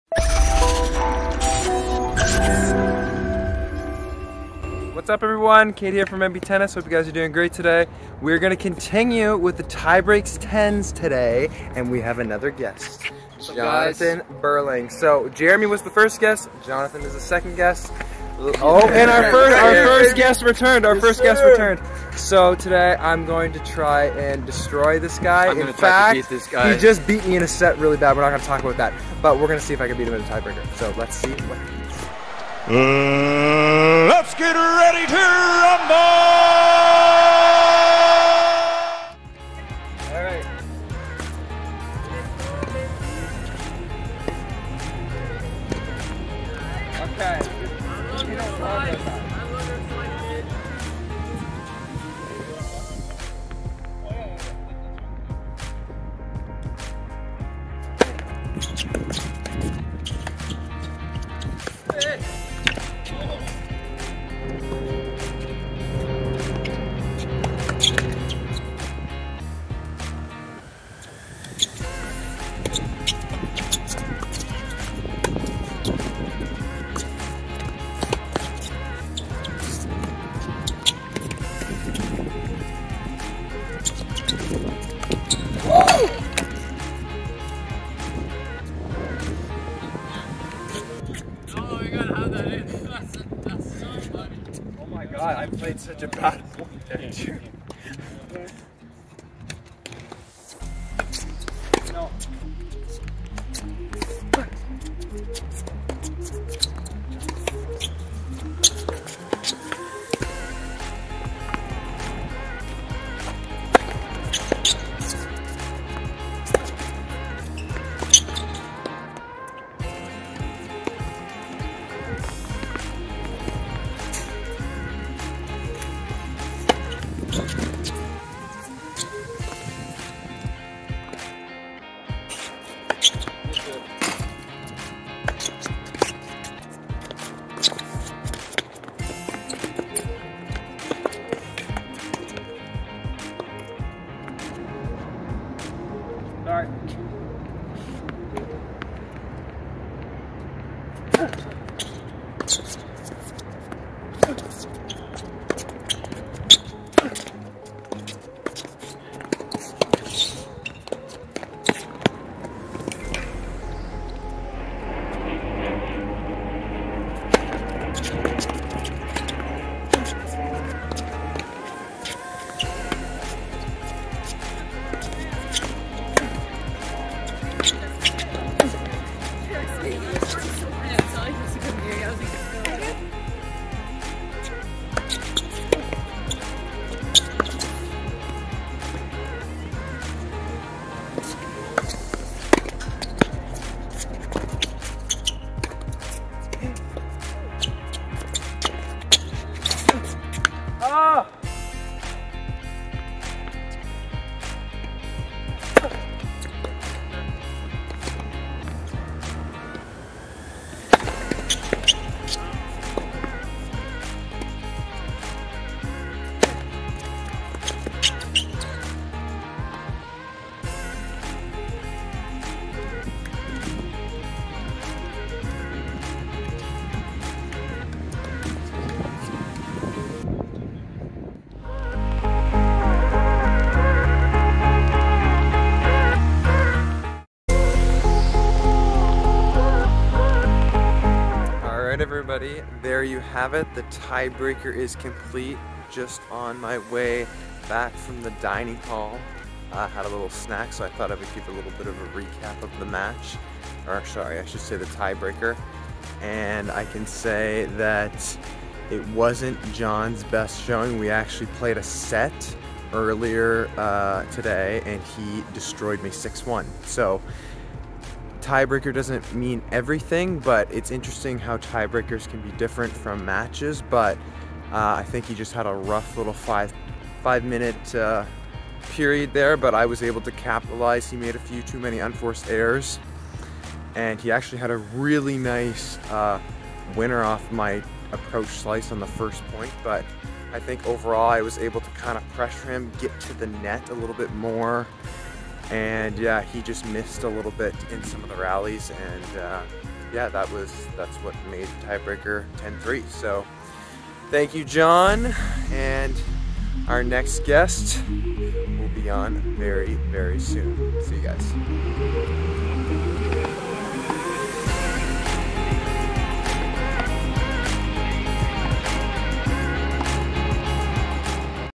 COLLEGE TENNIS: Tie Break 10's With SNHU Teammates | Match #2 Jonathan Berling From Norway
My second TieBreakTens match against my SNHU teammates! 🙌🙌 Today, I faceoff against our Number 1 player, from Norway, the Lefty Gunslinger, Jonathan Berling.

Tie Break 10's is a video series where I will play a 10 point tiebreaker against each one of my teammates.  You can watch the highlights, react to the match and see how my game matches up against each of them.

0:00 Intro
0:14 Meet My Opponent
0:31 Let's Get Ready To Rumble
3:52 Tiebreak Stats
4:01 Tiebreak Recap

Do I have what it takes to win? 🏆🏆

Father, son and daughter sharing our tennis journey, for other players, parents and coaches. We 0-0 Tennis ❤️🎾🇨🇦